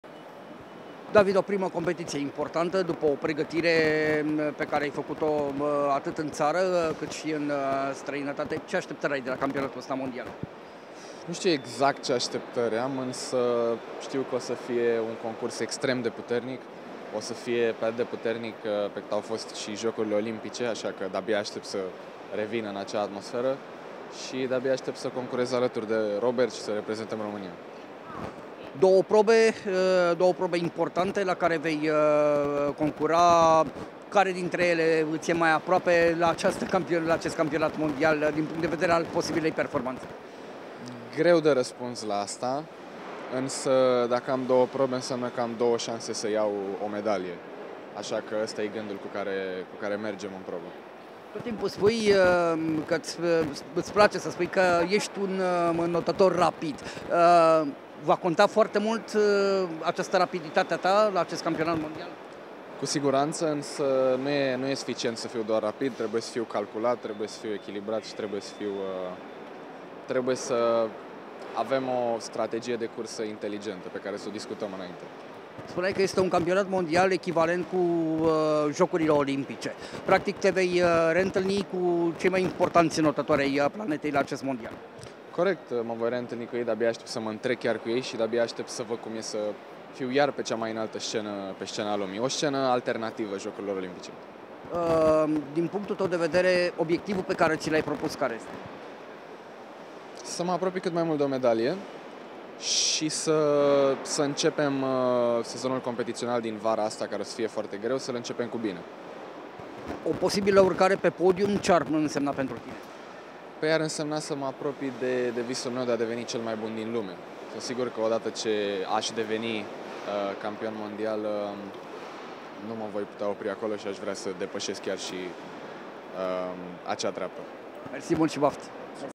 David Popovici, la plecarea la CM: "Dacă voi deveni campion mondial, nu mă voi putea opri acolo"
David Popovici, 17 ani, a devenit dublu campion mondial de seniori la competiția găzduită de Budapesta. La plecarea spre Ungaria, sportivul român deborda de optimism și avertiza că este doar începutul.